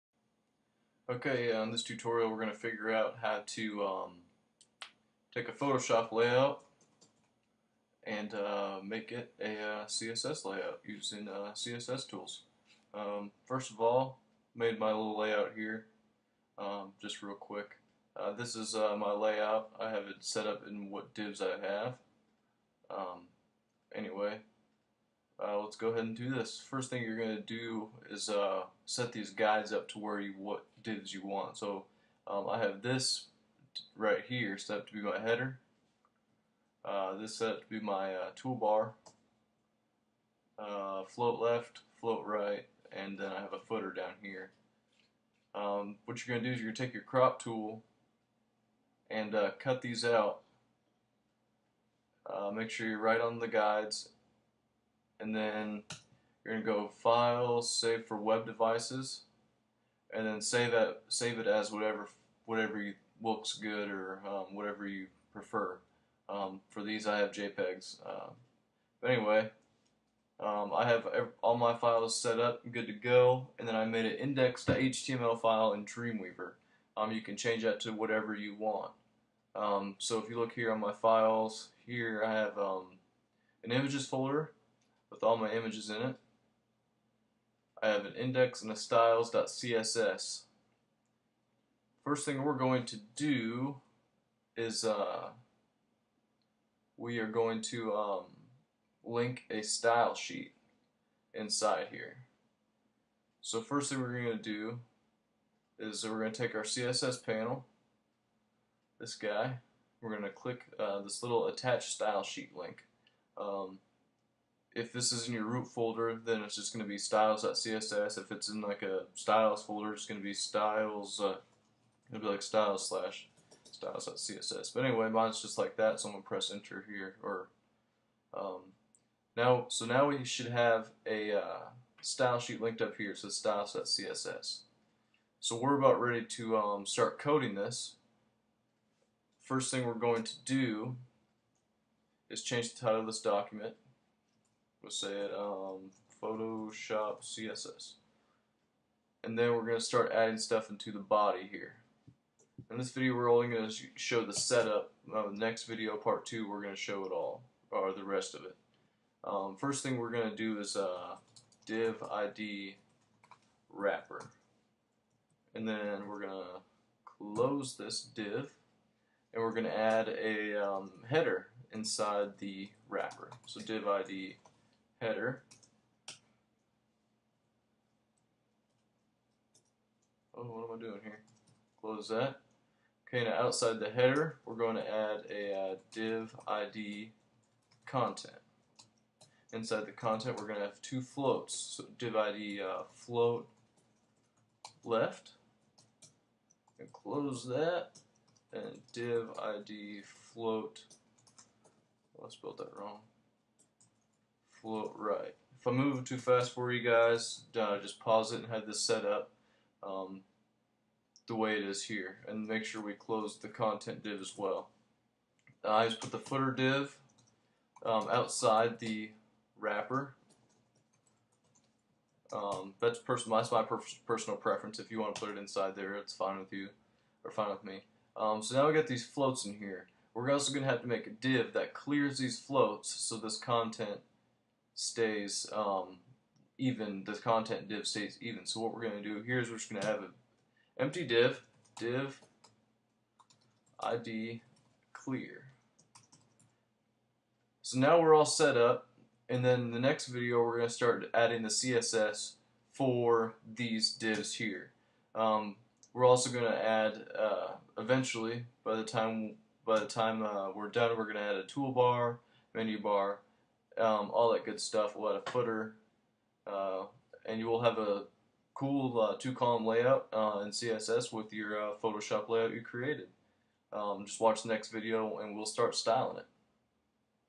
Part 1 How To: Create a website layout in Photoshop using CSS
In this video I will show you how to make that layout you made in photoshop work! Step By Step I will show you how to code your website using CSS.